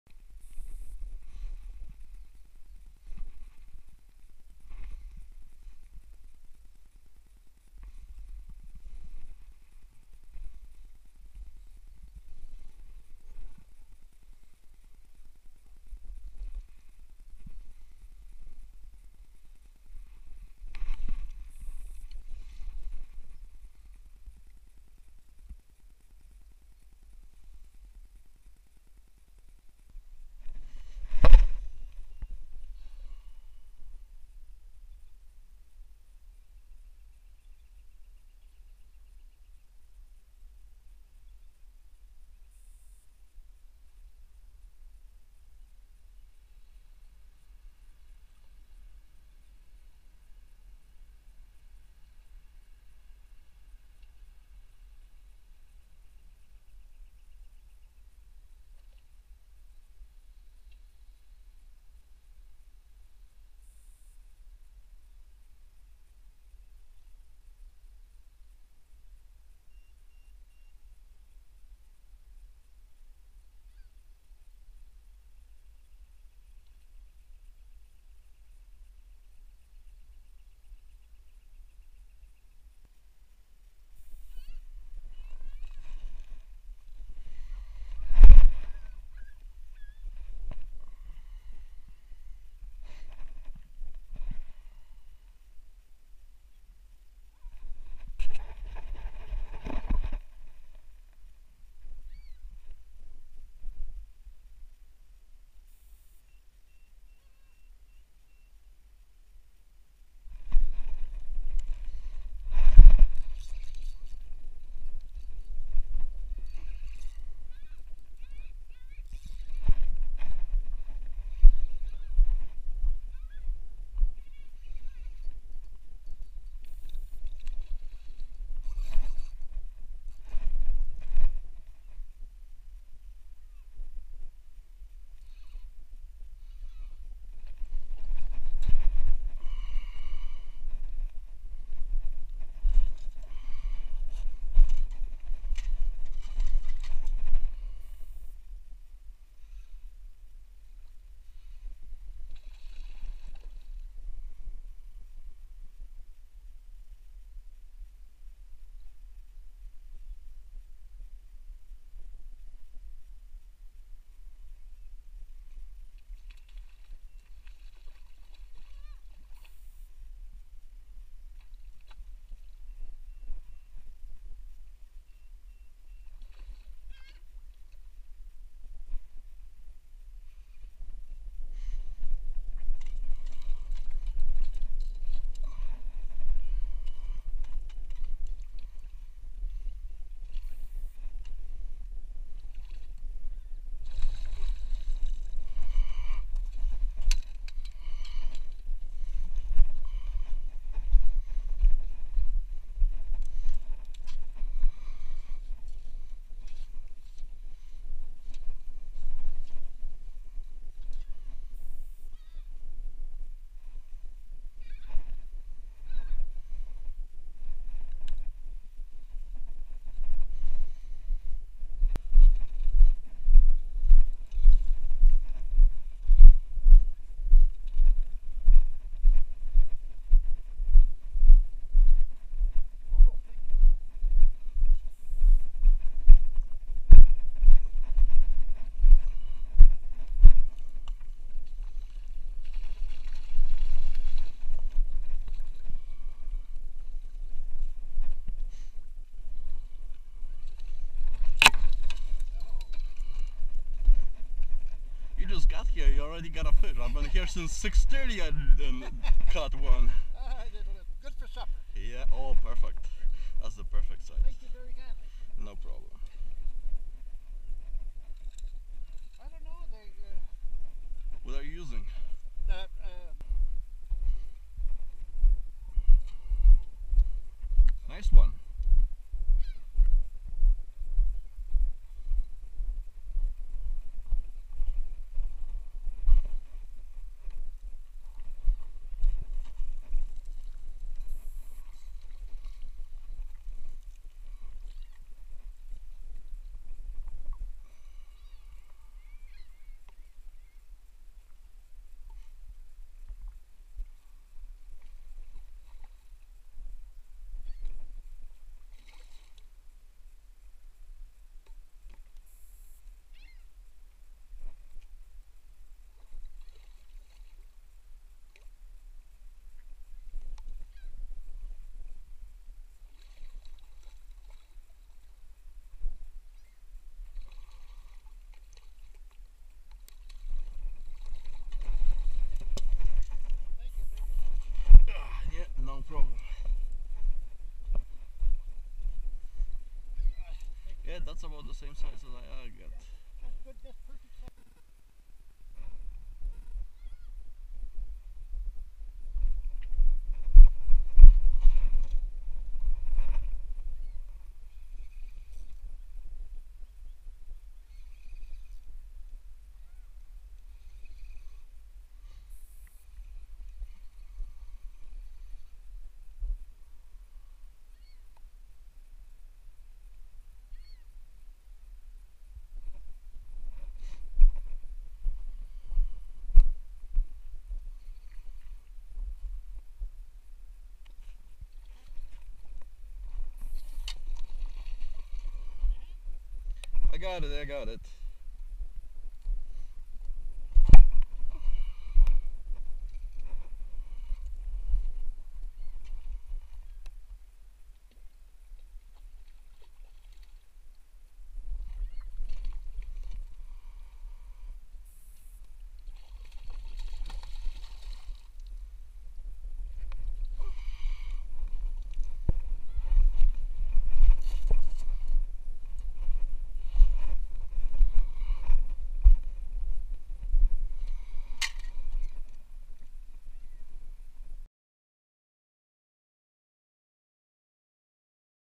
Rainbow Trout fishing on Parry Island, Ontario - POV fishing
Temperature: -2°C in the morning and 18°C at noon
Water depth: 12-18ft
Location: Parry Island
Bait used: trout dough (on Amazon Canada or Amazon US power eggs (on Amazon Canada or Amazon US

I arrived at the location at around 6:30am and was greeted by the puppies as seen in the video and while playing with them I missed the first bite.  The fish were taking chum pellets from the surface but the bites were sporadic and didn’t want to take bait.

I tried using different color silicone eggs at first because the first bite was on orange one.  Then, I put floating pellets on hook and let it float but that still didn’t work.  After trying pellets I switched to orange and then pink trout dough and I succeeded with the pink, which landed me my first rainbow at around 10am.

My neighbors were using Trout Nuggets and were also successful at landing fish.  My second fish also took the pink trout dough later in the day around 1pm after rotating the baits to see if they take anything else.